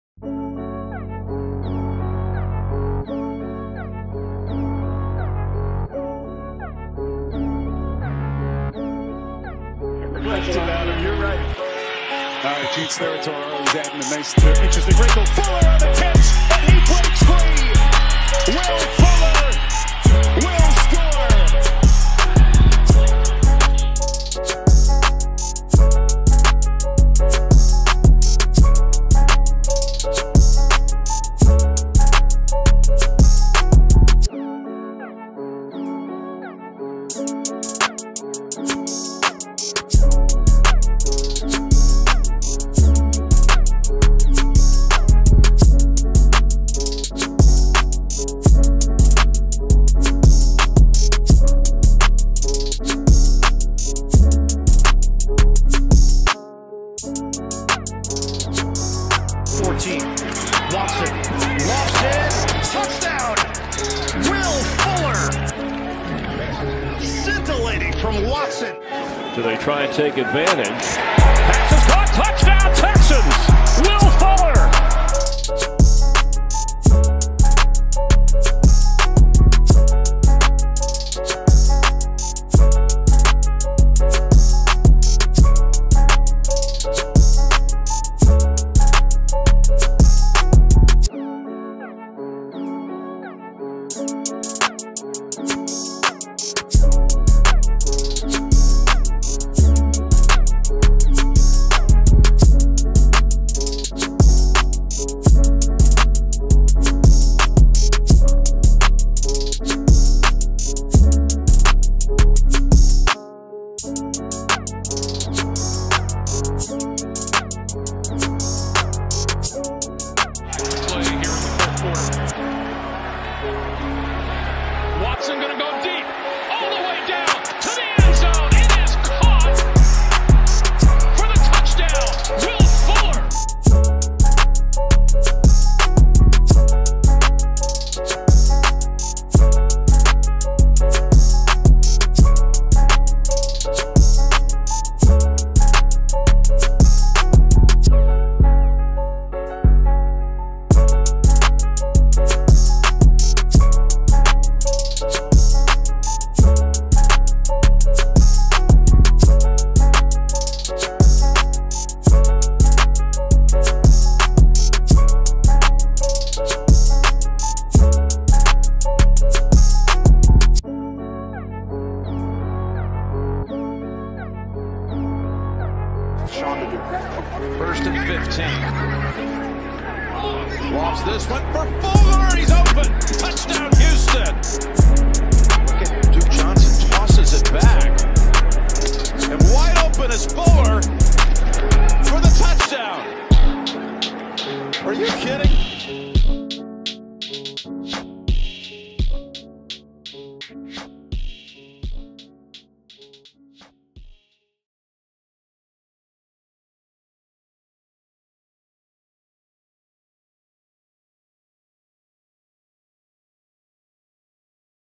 Will Fuller V 2020-2021 Highlights || Welcome to Miami
Will Fuller has signed with the Miami Dolphins, how will him and Tua pair together? In the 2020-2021 season, Fuller had:

53 Catches

879 Receiving Yards

8 Receiving Touchdowns

And 16.6 yards per catch